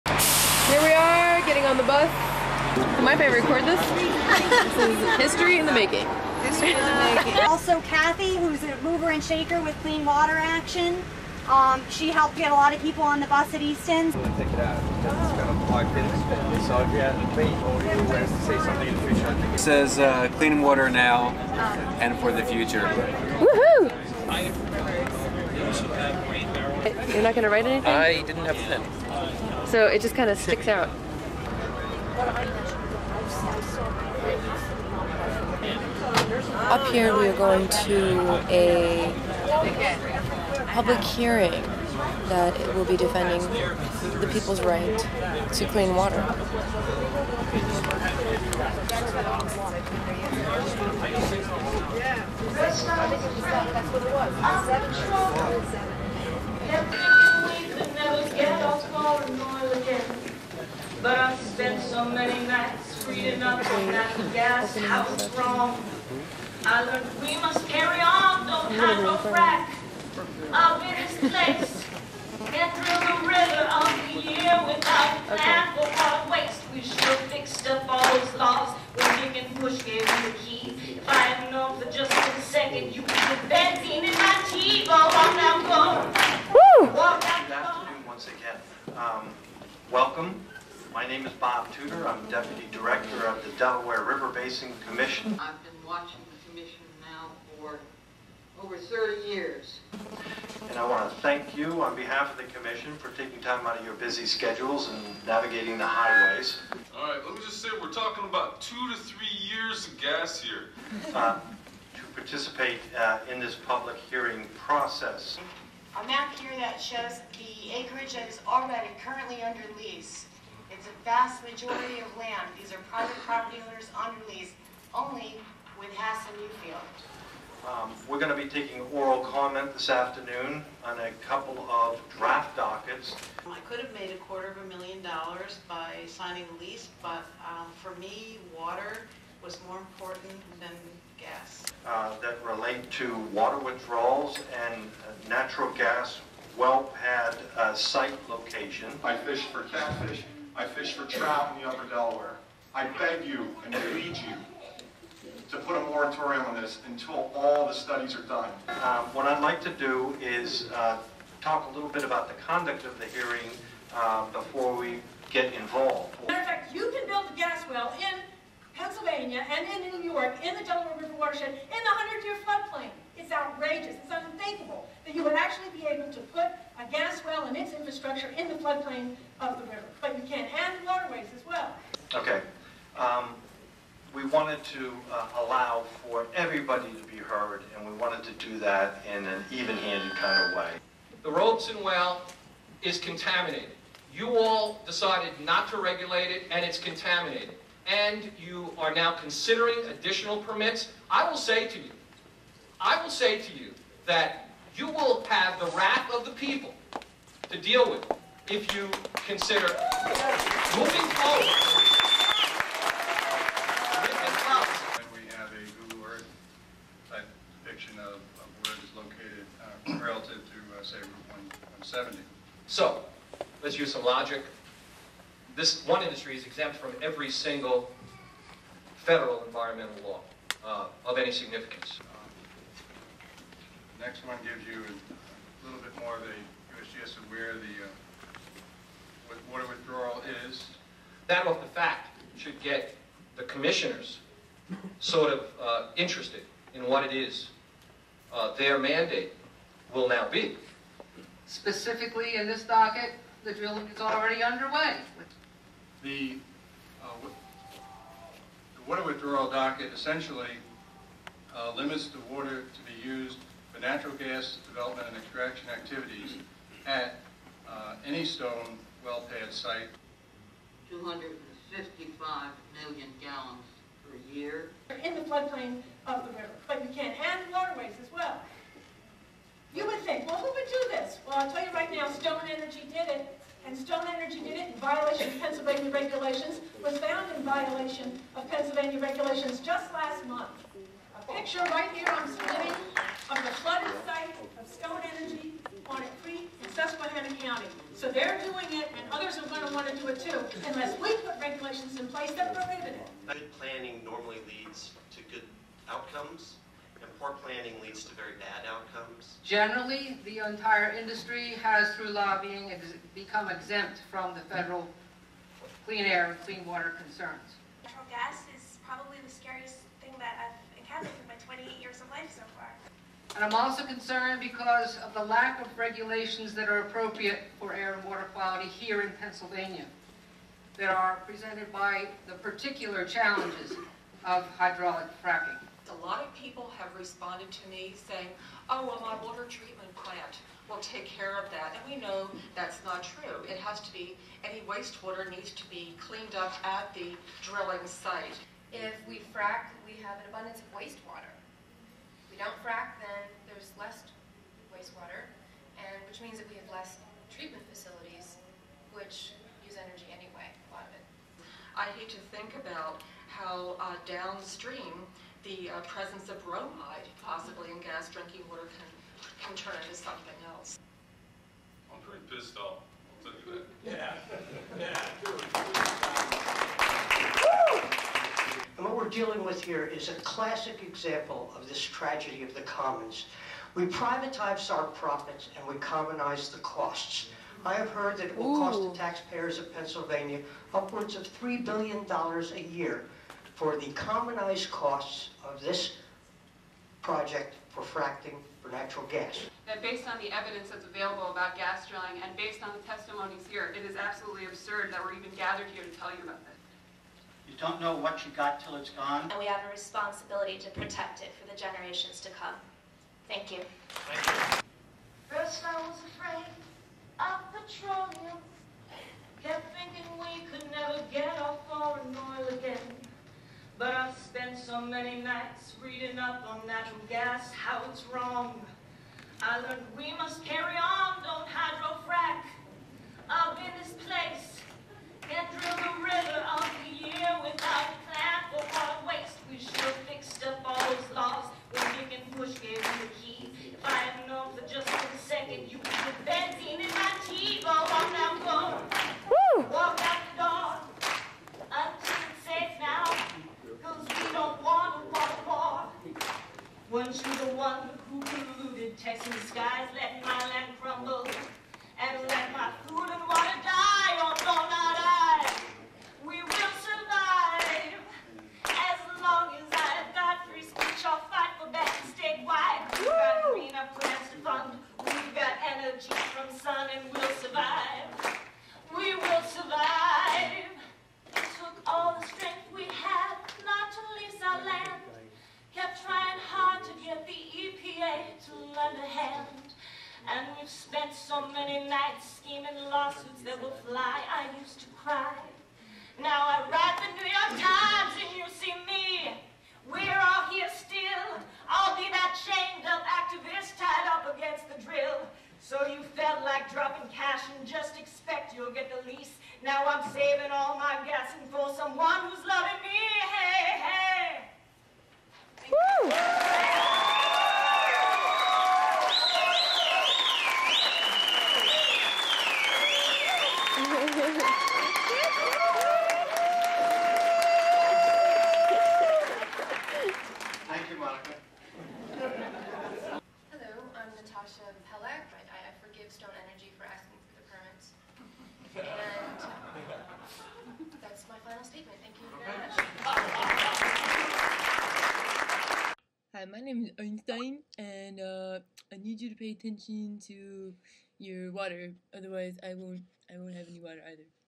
Water Wars
Your clean water is threatened by natural gas fracking.  Listen to the testimonies of the people downstream from the proposed drilling sites.

In connection with natural gas drilling, the commission has identified three major areas of concern:

Gas drilling projects in the Marcellus Shale or other formations may have a substantial effect on the water resources of the basin by reducing the flow in streams and/or aquifers used to supply the significant amounts of fresh water needed in the natural gas mining process.

On-site drilling operations may potentially add, discharge or cause the release of pollutants into the ground water or surface water.

The recovered "frac water" must be treated and disposed of properly.